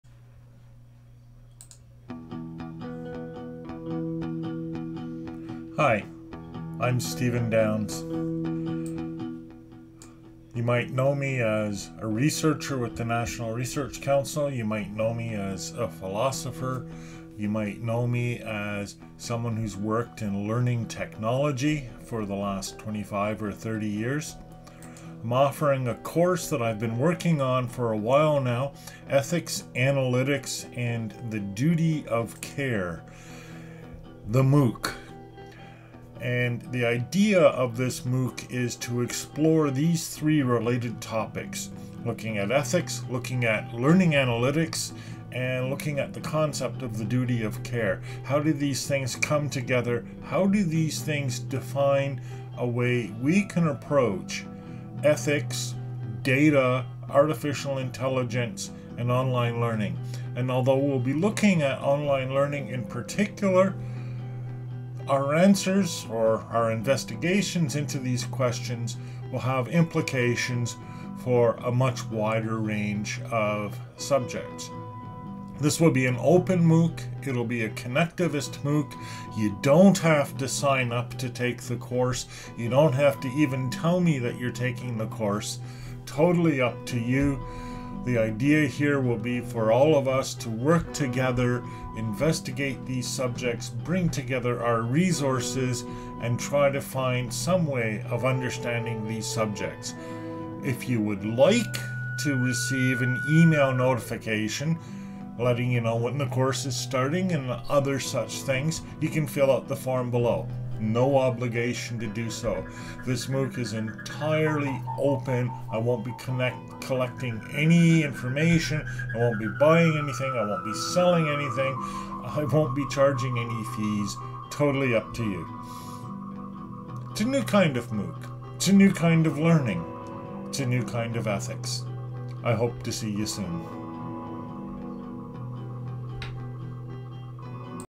Ethics, Analytics and the Duty of Care: The MOOC: The Intro
This 8-week MOOC will run during October and November, 2021, and will be facilitated by Stephen Downes.

Note that you do not have to register to take the course. This is completely optional, and I'm collecting email addresses only so I will know where to send you an email, if you want an email. You will be able to unsubscribe at any time.